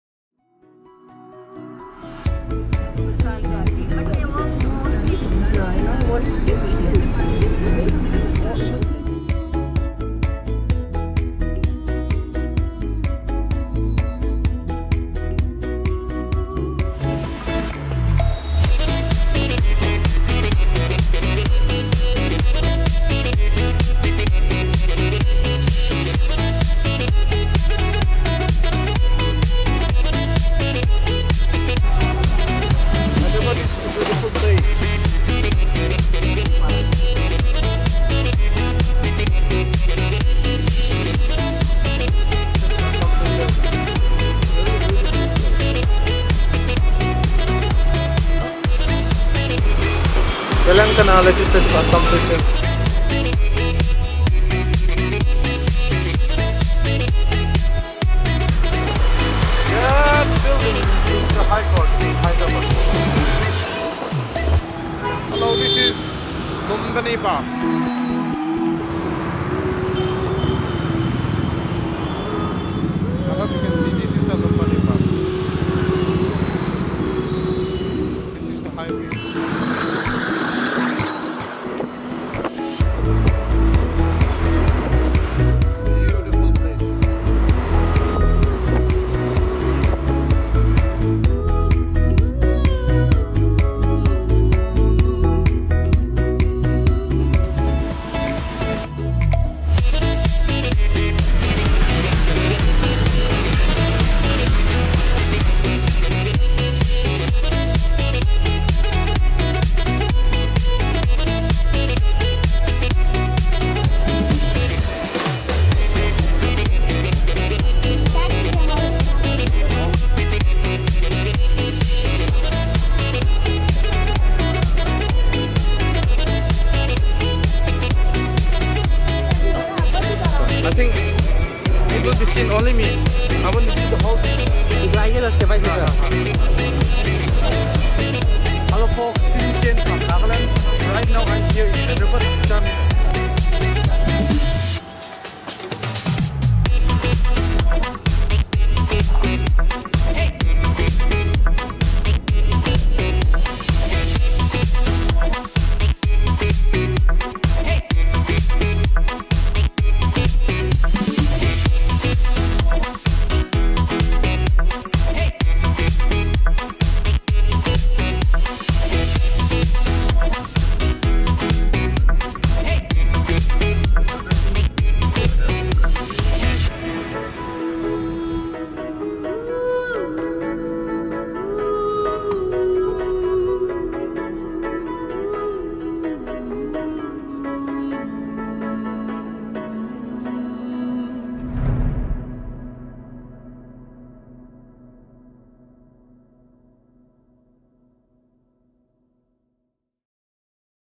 Travel video In India (Hyderabad) 2019 Charminar, Rajiv Gandhi International Airport.
Visited the most amazing places in Hyderabad like Charminar ( ''four minarates'' ) constructed in 1591 which is a monument and mosque, Legislative Assembly, High Court founded by the 7th Nizam H.E.H Mir Osman Ai Khan, Lumbini park named after Lumbini of Nepali dedicated to Lord Budha, Salar Jung Museum which is one of the three national Museum in India, and off course Rajiv Gandhi International Airport.